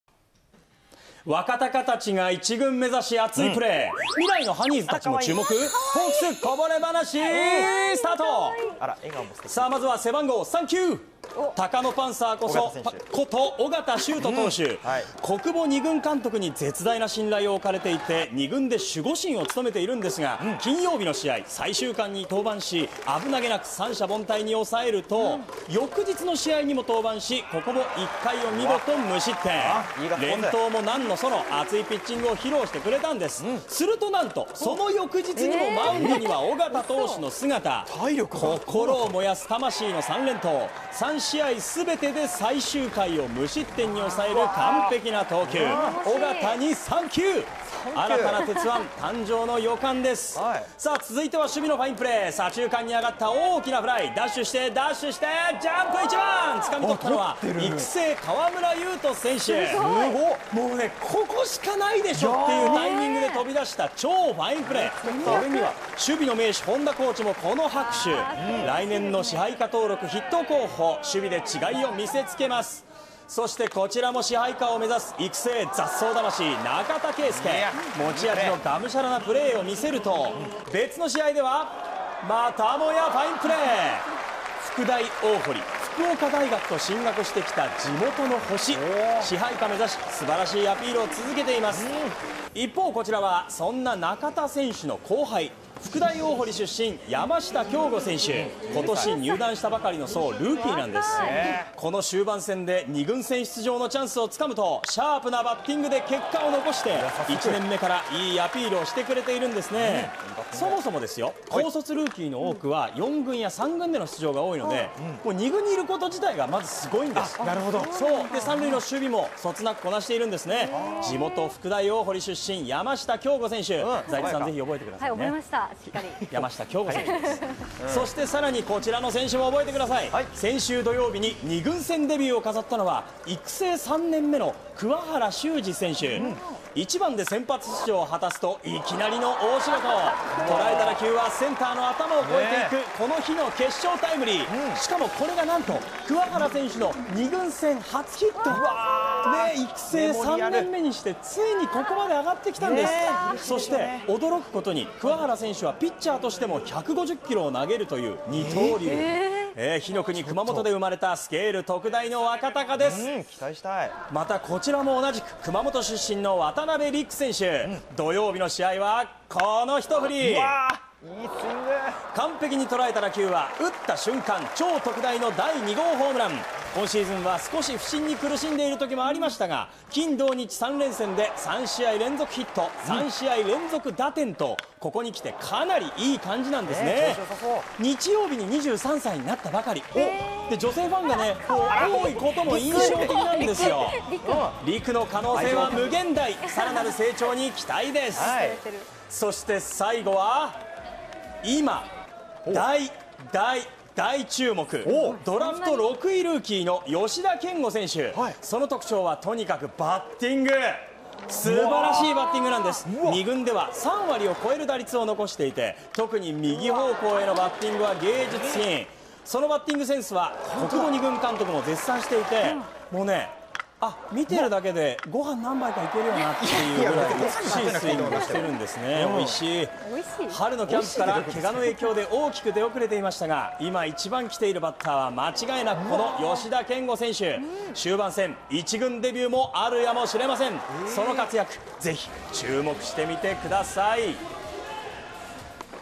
小久保２軍監督も大絶賛！！ドラ６ルーキー・吉田賢吾の非凡な打撃センス【シリスポ！ホークスこぼれ話】
３年ぶりのリーグ優勝間近のホークス２軍の若鷹軍団。

終盤に来て大注目は、ドラフト６位ルーキーの吉田賢吾選手。

春先はケガで出遅れましたが、９月は２軍の中心選手として大活躍。

非凡な打撃センスで魅せる、芸術的な右打ちは必見。

今季中の１軍デビューあるか？